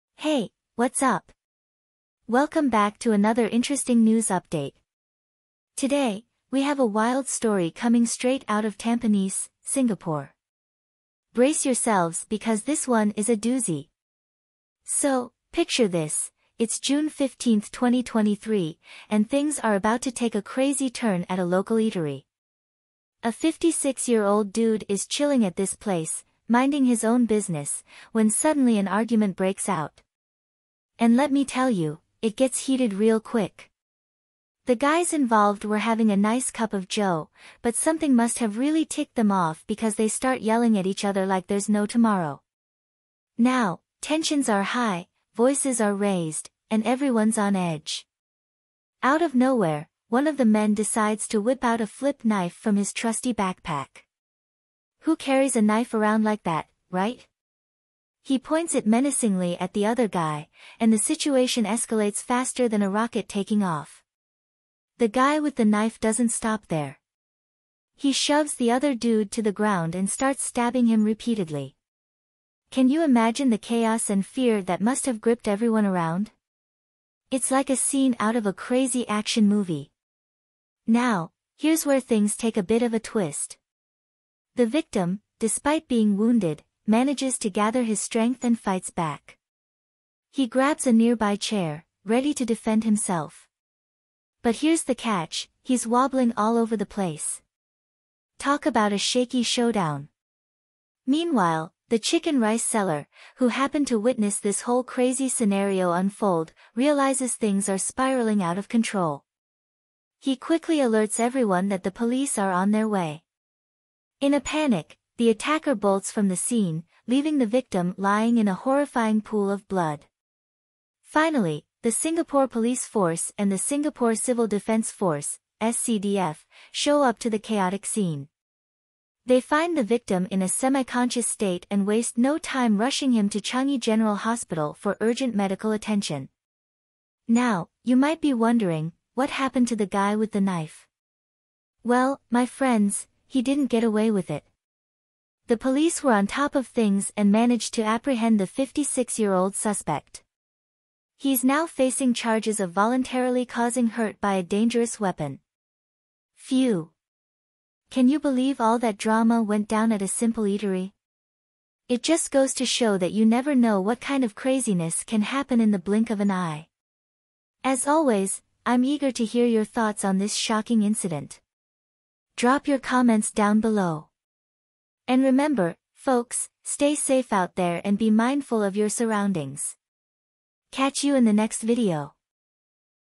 Chaos Unleashed: Knife-Wielding Man Attacks at Tampines Food Stall
On June 15, 2023, a 56-year-old man was arrested at a Tampines eatery in Singapore after allegedly stabbing a 57-year-old man. The police received a call for assistance at 12:40 PM at Block 513 Tampines Central, and the Singapore Civil Defence Force (SCDF) was alerted to the incident around 12:45 PM.

According to reports from The Straits Times, a heated argument broke out between the two men at around 12 PM while they were drinking coffee. The argument escalated, and one of the men reportedly pulled out a flip knife from his backpack and pointed it at the other person. The man with the knife pushed the other man to the ground and stabbed him multiple times. The victim managed to get up and attempted to defend himself by picking up a chair but was apparently unsteady.